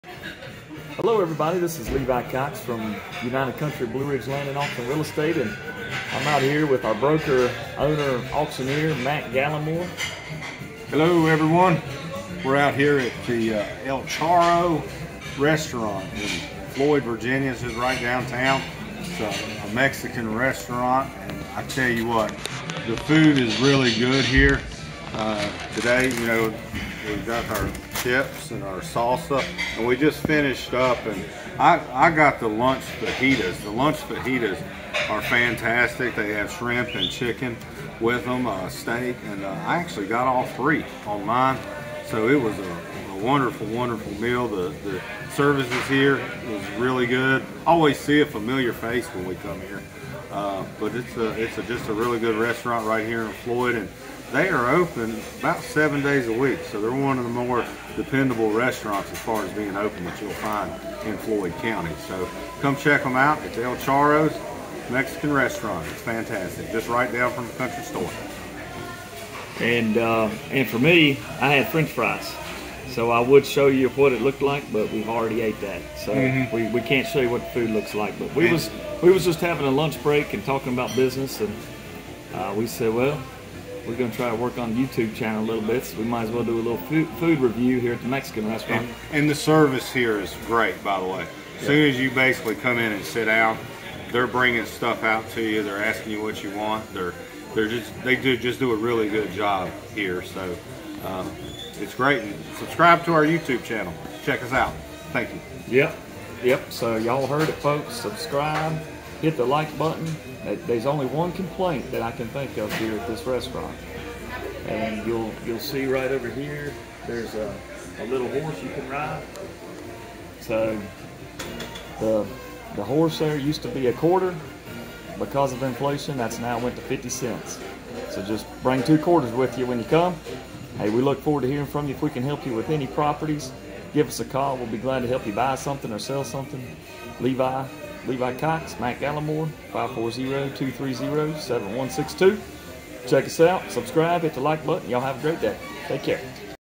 El Charro Mexican Grill Review - Floyd VA - Adventures of Matt & Levi
Matt Gallimore and Levi Cox enjoy lunch at El Charro Mexican Grill in Floyd VA. It is a great choice when you visit Floyd VA!